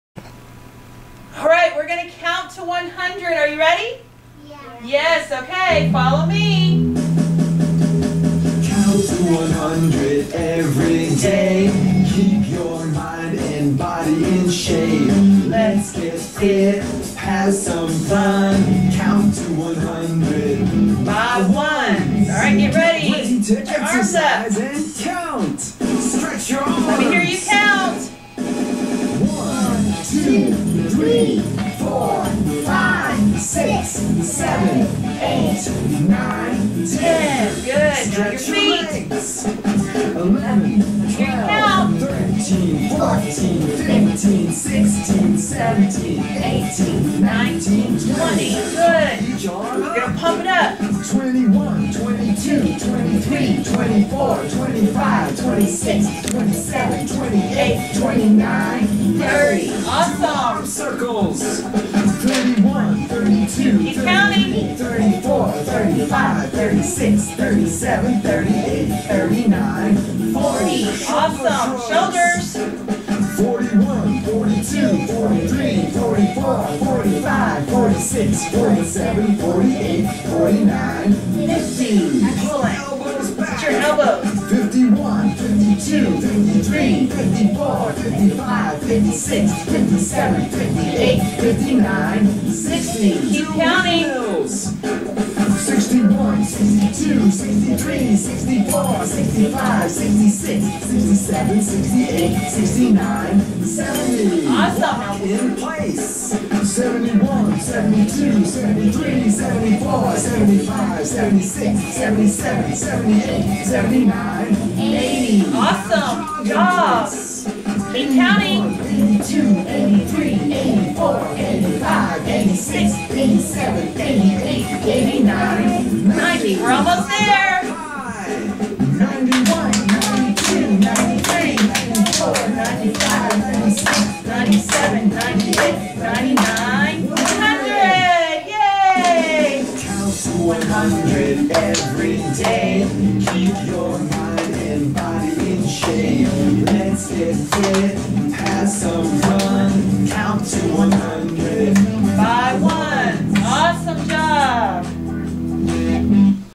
Hemphill Counting to 100
In this segment, students practice counting to 100 with various exercises. Hemphill math intervention teacher Samantha Baas leads students in this fun integration of math, music, and movement. Perfect for pre-k, kindergarten, first grade, and intervention students who need practice with counting.